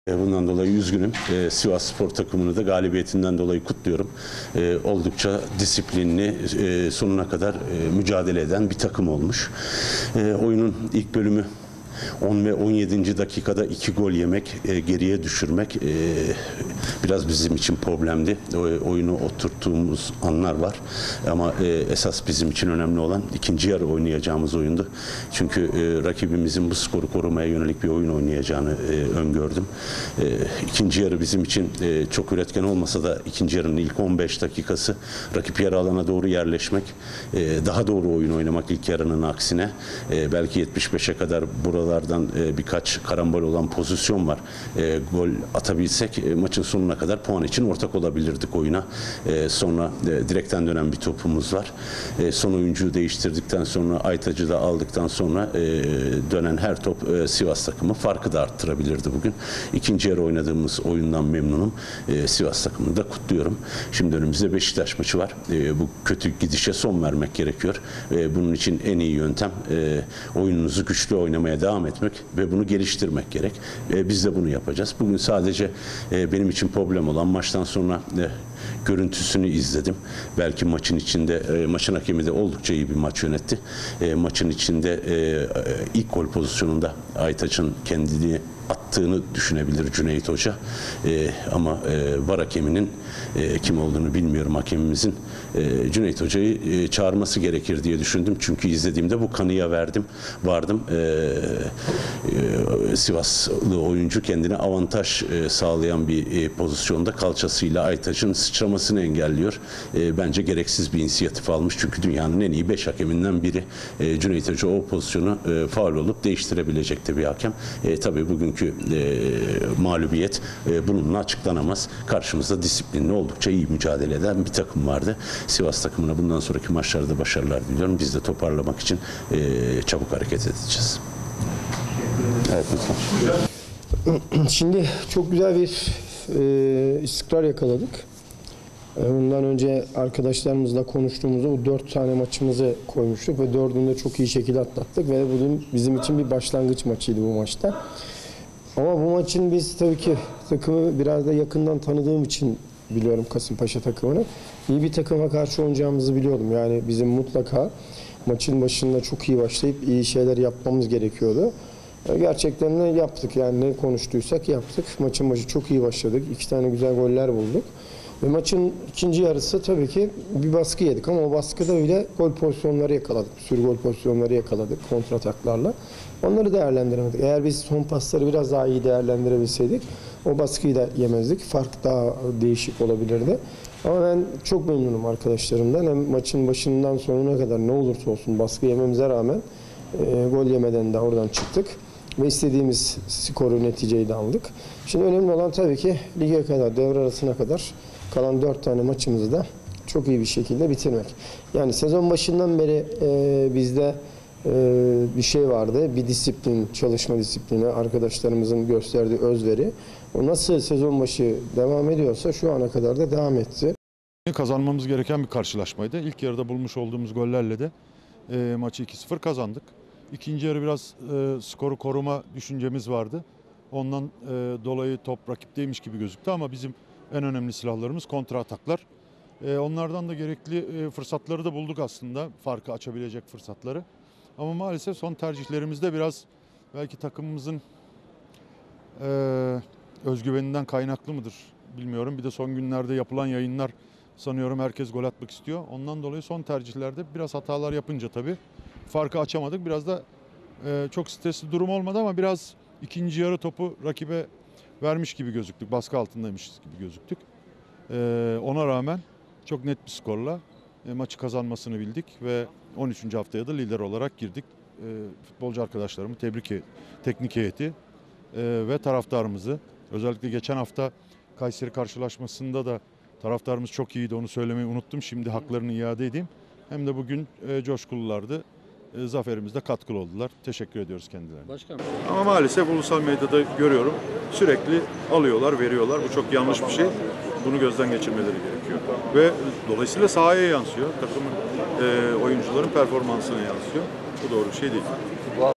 Sivasspor - Kasımpaşa maçının ardından
Sivasspor Teknik Direktörü Rıza Çalımbay, galibiyetten dolayı mutlu olduklarını belirterek, "Maç öncesi konuştuğumuz her şeyi yaptık. Biraz dikkatli olsak sonuç farklı olabilirdi" dedi. Rakibi kutlayan Kasımpaşa Teknik Direktörü Kemal Özdeş, "İkinci yarı gol bulabilsek maçın sonuna kadar puana ortak olabilirdik" ifadelerini kullandı. Takımı kutlayan Demir Grup Sivasspor Başkanı Mecnun Otyakmaz ise futbolcuları hakkında basında çıkan transfer haberlerine tepki gösterdi.

Demir Grup Sivasspor Teknik Direktörü Rıza Çalımbay, maçta istedikleri her şeyi doğru yaptıklarını belirterek, "Çok güzel bir istikrar yakaladık. Bundan önce arkadaşlarımızla konuştuğumuzda 4 maç koymuştuk ve 4'ünü dü çok iyi şekilde atlattık. Bugün bizim için de bir başlangıç maçıydı bu maç. Bu maçta Kasımpaşa takımını biraz da yakından tanıdığım için biliyorum, iyi bir takıma karşı oynayacağımızı biliyorduk. Bizim mutlaka maçın başında çok iyi başlayıp, iyi şeyler yapmamız gerekiyordu. Gerçekten de ne konuştuysak yaptık. Maçın başı çok iyi başladık, 2 tane güzel gol bulduk. Maçın ikinci yarısı tabii ki baskı yedik ama o baskıda bile gol pozisyonları yakaladık kontra ataklarla. Bunları değerlendiremedik. Eğer biz pasları biraz daha iyi değerlendirebilseydik o baskıyı da yemezdik, fark daha değişik olabilirdi. Ama ben çok memnunum arkadaşlarımdan. Maçın başından sonuna kadar, ne olursa olsun, baskı yememize rağmen gol yemeden buradan çıktık ve istediğimiz skoru ve neticeyi de aldık. Şimdi önemli olan devre arasına kadar kalan 4 tane maçımızı da çok iyi bir şekilde bitirmek. Yani sezon başından beri bir şey vardı. Bir çalışma disiplini, arkadaşlarımızın göstermiş olduğu özveri. Nasıl sezon başı devam ediyorsa şu ana kadar da devam ediyor. Hepsi de kapasitelerinin üstüne çıktılar. Hep söylüyorum, bizim en iyi iki tane transferimiz Erdoğan ve Mert Hakan'dır. Gerçekten onları da kazandırdık. İyi oynuyorlar, çok iyi oynuyorlar. Diğer arkadaşlarımızın da hepsi performansının bayağı üstüne çıkıyorlar. İnşallah sonuna kadar devam ettirmeye çalışacağız. Tabii ki kolay değil. Ligdeki puanlar birbirine yakın. İki tane galibiyet aldığında iyi bir yere geliyorsun. Tersi olduğu zaman istemediğin bir yere gelebiliyorsun. Hepsinin farkındayız ve bu iş çalışmadan olmuyor. Biz de yakaladığımız bu istikrarı sonuna kadar devam ettirmek için ne gerekiyorsa hepsini yapacağız. Çünkü burada iyi bir arkadaşlık ortamı var, iyi bir yönetim var. Taraftarımız da takımla bütünleşti. Güzel bir ortam yakalandı. Önemli olan tabii ki sakatsız, cezasız geçebilirsek bu maçlarımızı iyi bir şekilde de ilk yarıyı bitirebileceğimizi düşünüyorum" dedi.

ÖZDEŞ: GOL ATSAK PUANA ORTAK OLABİLİRDİK

Kasımpaşa Teknik Direktörü Kemal Özdeş ise Sivasspor'u galibiyetinden dolayı kutlayarak, "Bugün 2-0 kaybettik. Bundan dolayı üzgünüz. Sivasspor'u galibiyetinden dolayı kutluyorum. Oldukça disiplinli, sonuna kadar mücadele eden bir takım olmuş. Oyunun ilk bölümü 10 ve 17'nci dakikalarda 2 gol yiyip geriye düşmek problemdi. Bizim için önemli olan ikinci yarı oynayacağımız oyundu. Çünkü rakibin ikinci yarı skoru korumaya yönelik oynayacağını öngördük. İkinci yarı bizim için çok üretken olmasa da rakip ilk 15 dakika rakip yarı alana yerleşmek ilk yarının aksine daha doğru oyun oynamaktı. Karambol pozisyonlar var. Gol atabilsek maçın sonuna kadar puana ortak olabilirdik. Direkten dönen topumuz var. Son oyuncu değiştirdikten sonra dönen her topta Sivas takımı farkı da artırabilirdi. İkinci yarıdaki oyundan memnunum. Sivas takımını da kutluyorum. Şimdi önümüzde Beşiktaş maçı var. Bu kötü gidişe son vermek gerekiyor. Bunun için en iyi yöntem oyununuzu güçlü oynamaya devam etmek, bunu geliştirmek. Biz de bunu yapacağız. Bugün sadece benim için problem olan maçtan sonra da görüntüsünü izledim. Belki maçın içinde hakem de oldukça iyi bir maç yönetti. İlk gol pozisyonunda Aytaç'ın kendini attığını düşünebilir Cüneyt hoca. Ama VAR hakeminin Cüneyt hocayı çağırması gerekir diye düşündük,. İzlediğimde bu kanıya vardım. Sivaslı oyuncu kendine avantaj sağlayıp pozisyonda Aytaç'ın sıçramasını engelliyor. Cüneyt hoca inisiyatif aldı. Ama bugünkü mağlubiyet bununla açıklanamaz. Karşımızda disiplinli, oldukça iyi mücadele eden bir takım vardı. Sivas takımına bundan sonraki maçlarda başarılar diliyorum. Biz de toparlamak için hareket edeceğiz" diye konuştu.